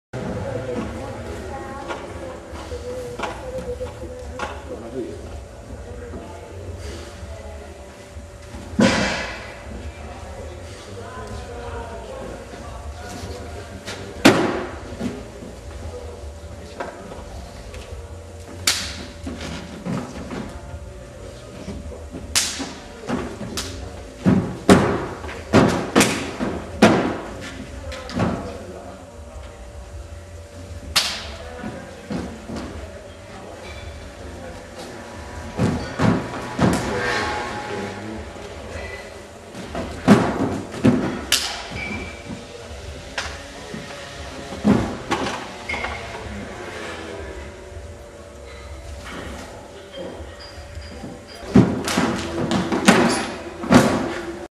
Esibizione Tang Lang (old) 2007 - Michele Batazzi
Forma dimostrativa Tang Lang (mantide) anno 2007